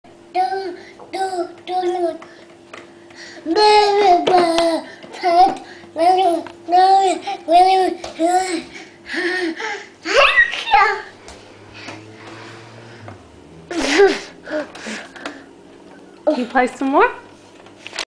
Joseph playing the tins
Getting percussive in the pantry